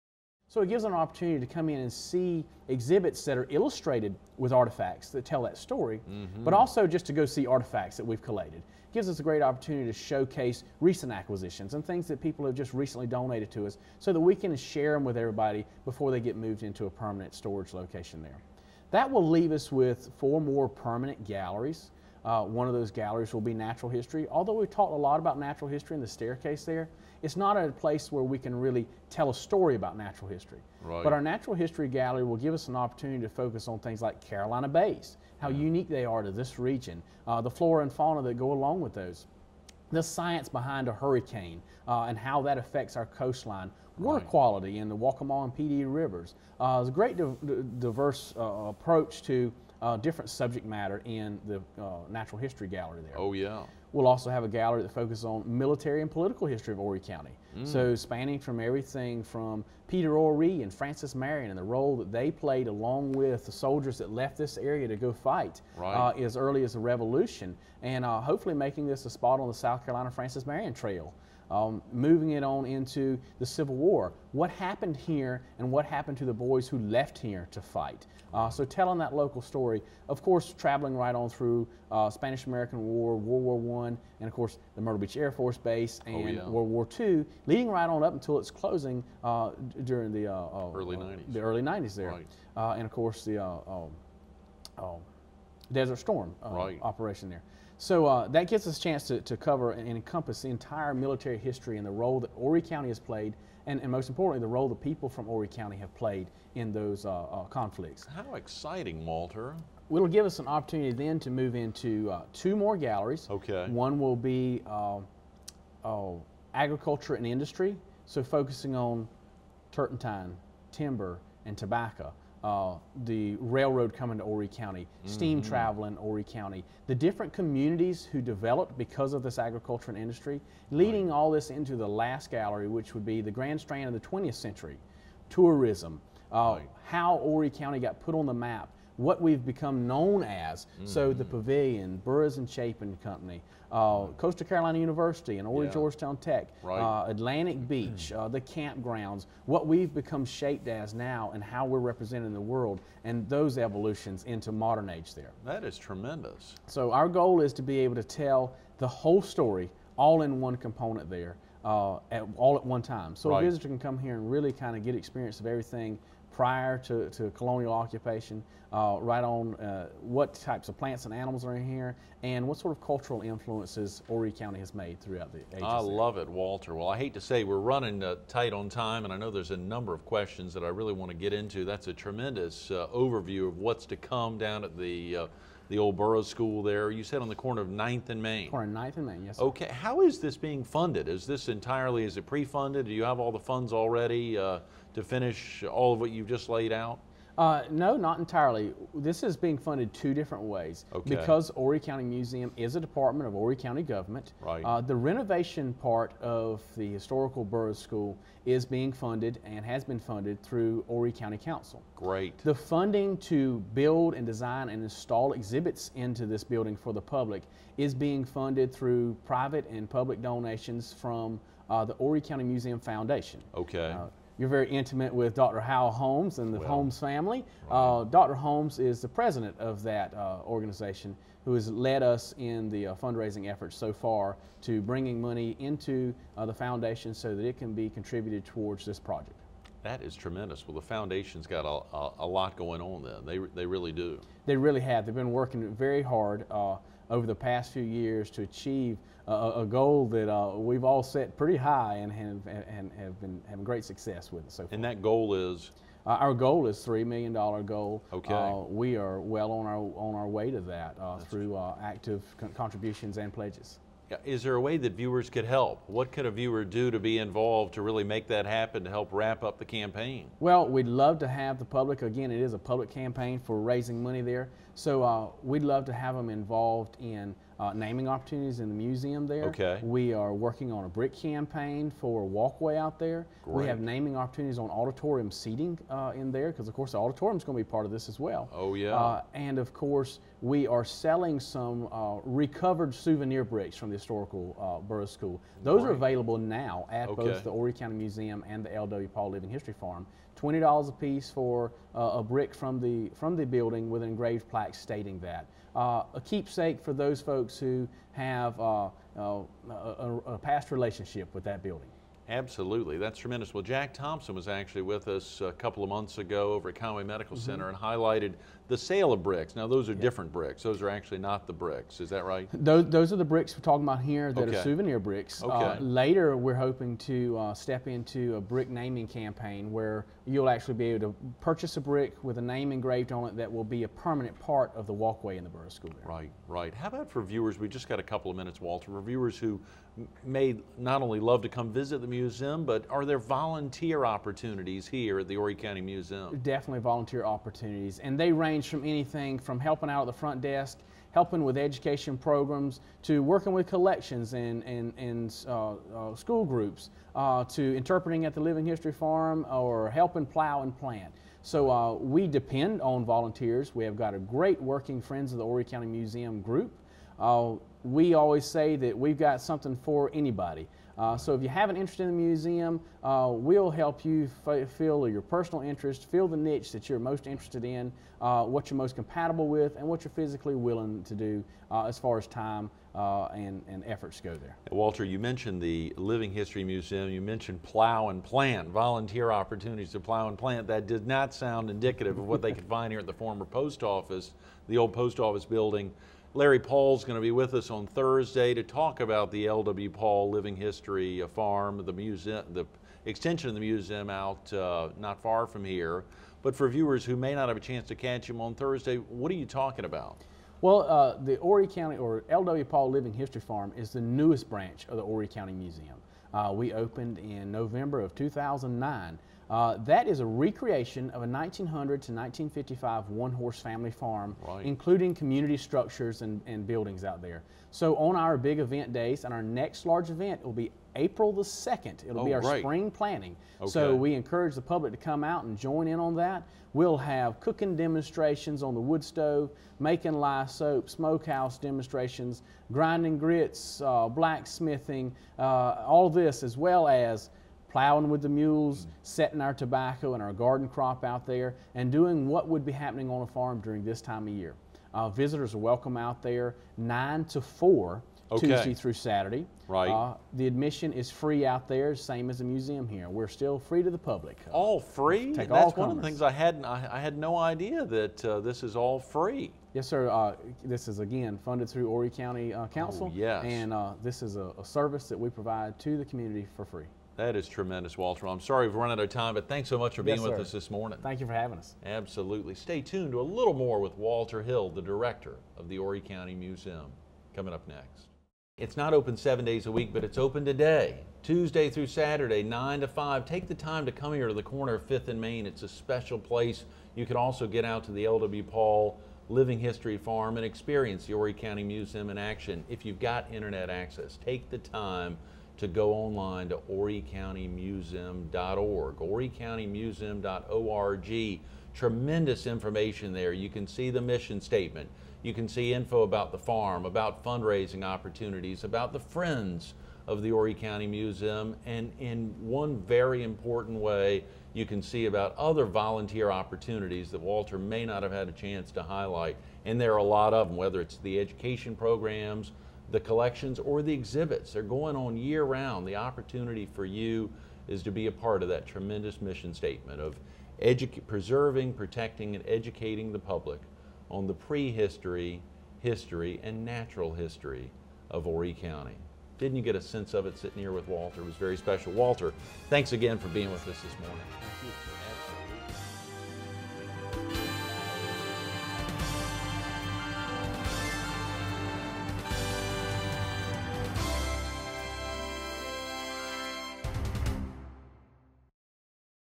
Show March 22, 2011 part 2
He's been fascinated by history since childhood and has a love of Horry County.
Who better to head up its museum?  We visit today with Walter Hill, Director of The Horry County Museum.  We learn the origins of the museum as well as the exciting plans for growth in its future.